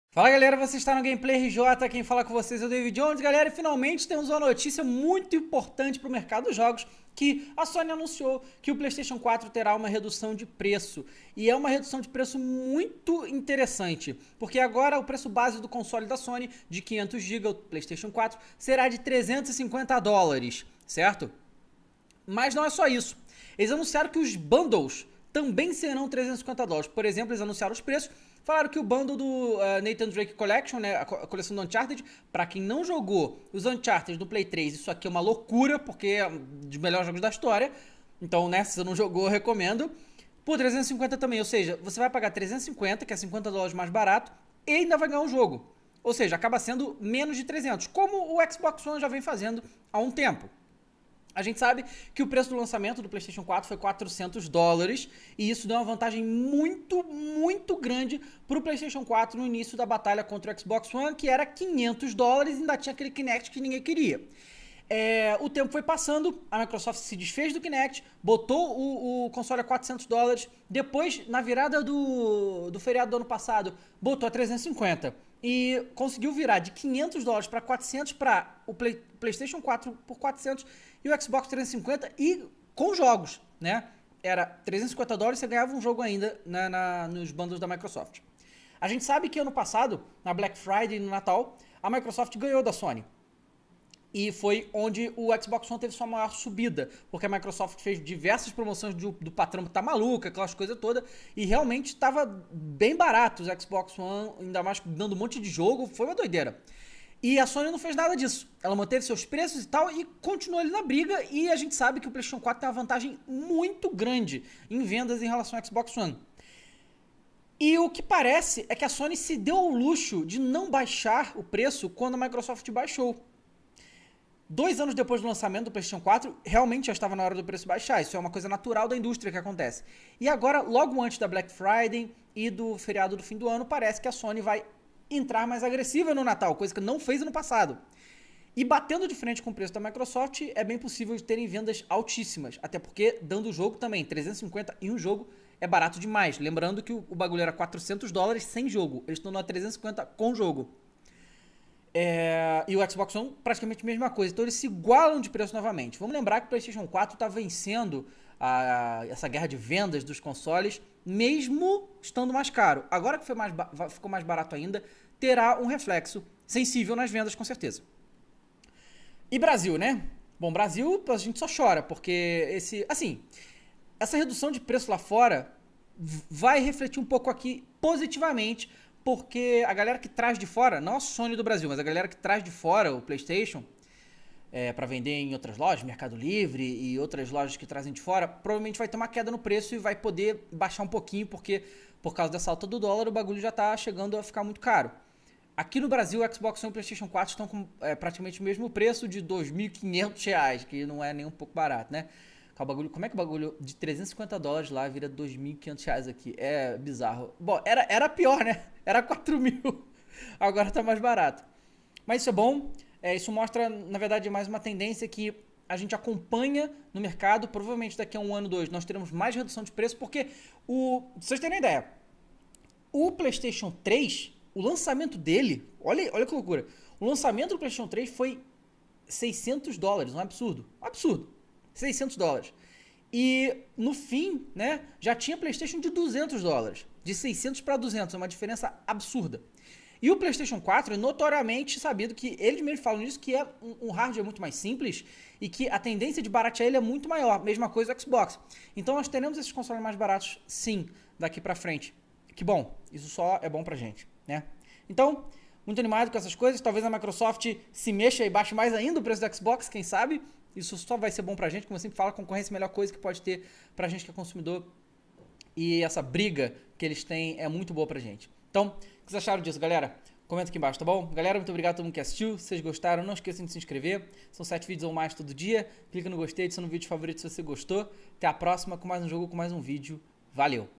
Playstation 4 tem corte de preços FINALMENTE, qual é o impacto disso?
Sim, finalmente o PS4 terá uma redução no seu preço! A Sony anunciou essa, que pra muitos, era uma notícia muito esperada!

Os Piratas